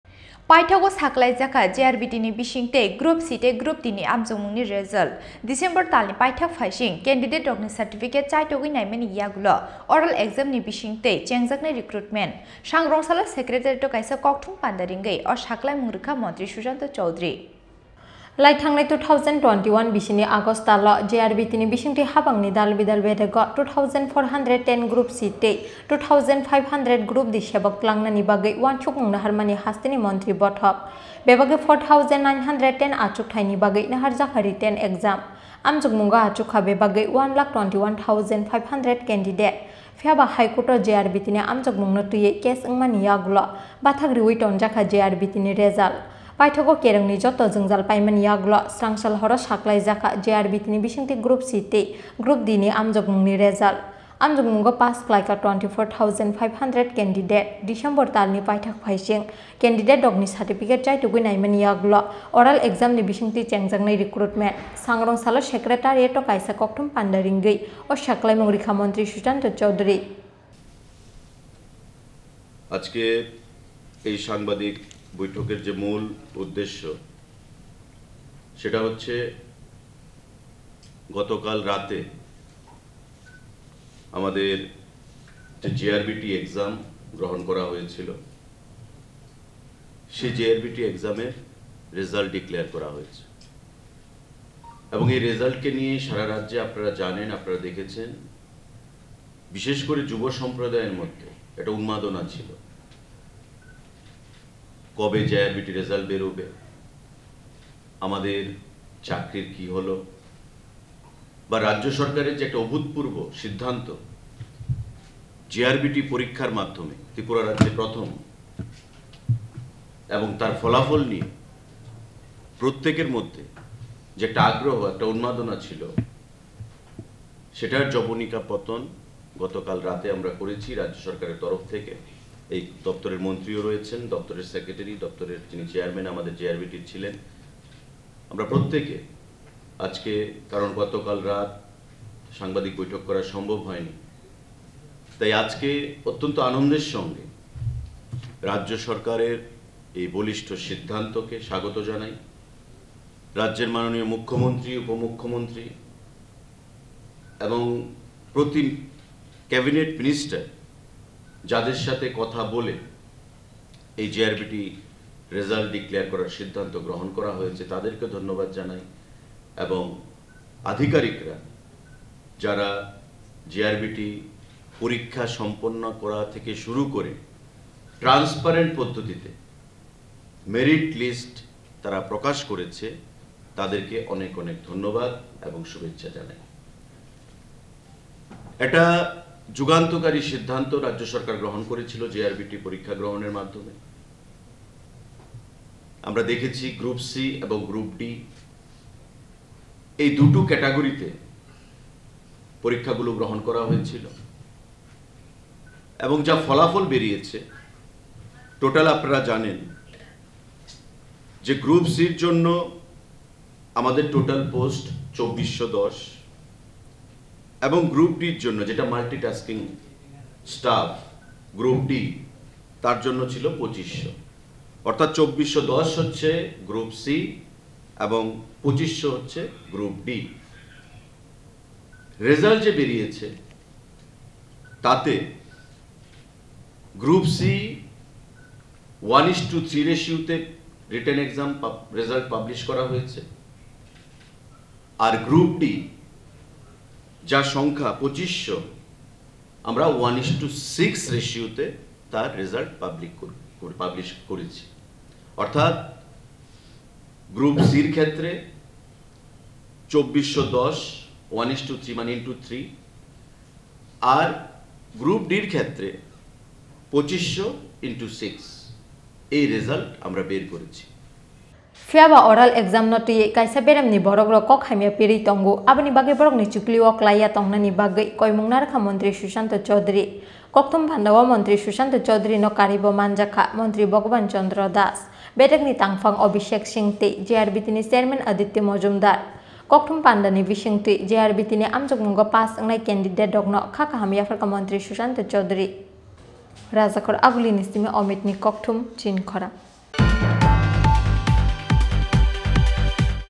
JRBT ni Examno twiwi Tatal Koktwma Pirwi Tongo Kaisa Beremni Bororok : Sushanta Chowdhury
🟢 Paithago Saklaijakha JRBT ni Bisingtwi Group-C tei Group-D ni Anjokmungni Result. December Talni Paithak Phaising, Candidate Rokni Certificate Chaitogwi Naimani Yakulo, Oral Exam ni Bisingtwi Chengjaknai Recruitment. Sangrongsalo Secretariat oh Kaisa Kokthum Panda Ringwi oh Saklaimung Rwkha Mantri Sushanta Chowdhury.